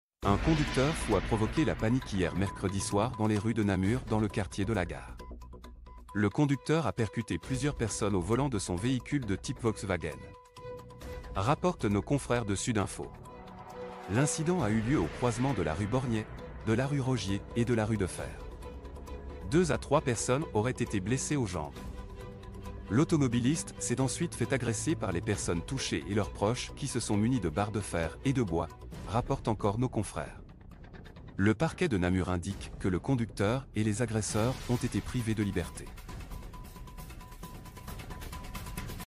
Un chauffard renverse des piétons à Namur puis se fait agresser à la barre de fer
- Un chauffard a renversé des piétons à Namur.
- Le chauffard a ensuite été agressé à la barre de fer.
- Les piétons ont été blessés mais leurs jours ne sont pas en danger.
- Le chauffard a été arrêté et placé en garde à vue.